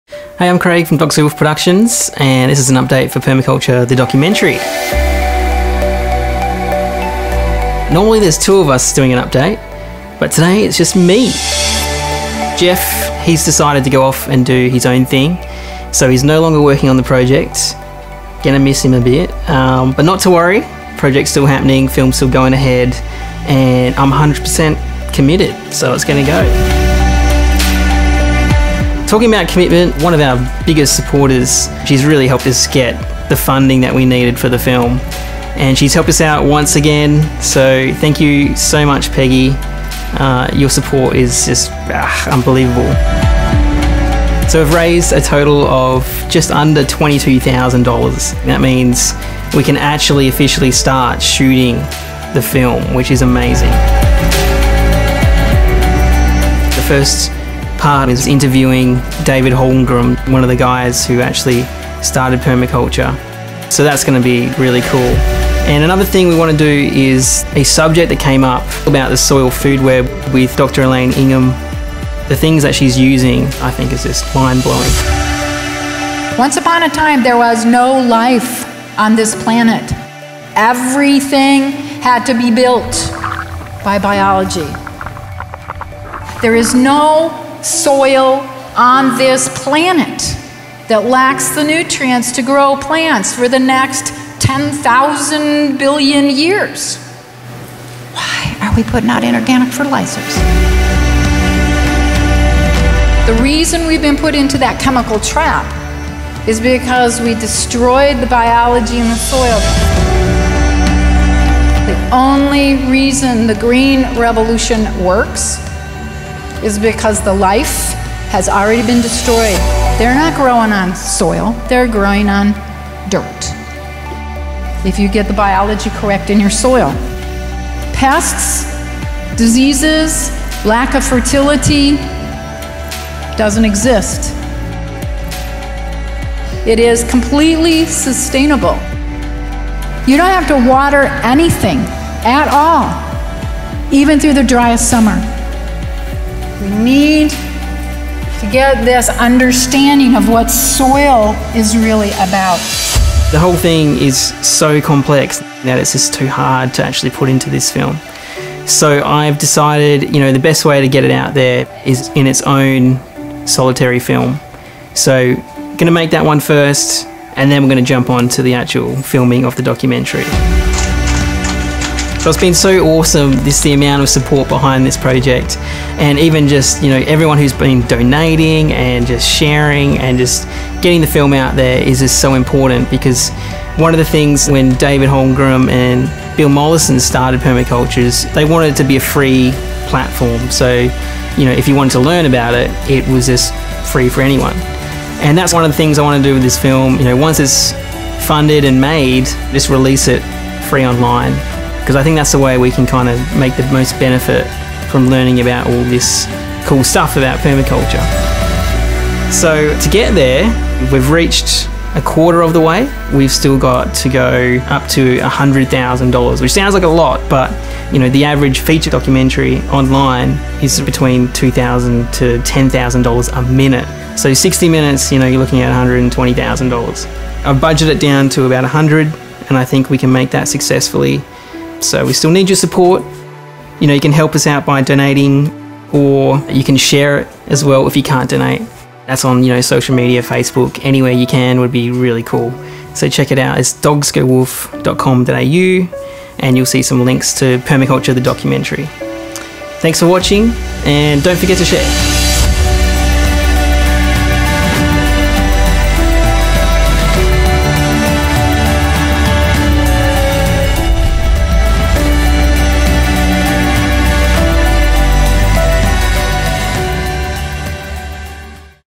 Permaculture The Documentary: Update 2019
Craig shares the new plans in store for 2019 about creating the film Permaculture The Documentary.

Geoff the producer of the doco has decided to pursue other ventures, leaving Craig to making the film, but he is confident the film will still be produced even running solo.

Another huge donation has bumped the funds up to a quarter of the end goal to make the film, which means officially shooting can begin! Massive thank you to Peggy!!

Two projects are in the works. One: to interview the remanning founder of Permaculture, David Holmgren.
Two: To create a short doco on the Soil Food Web with Dr. Elaine Ingham.

With only a quarter of the funds raised, we still need your help. You can help by donating or sharing. The bigger the community, the more people can help make changes that benefit us and the world.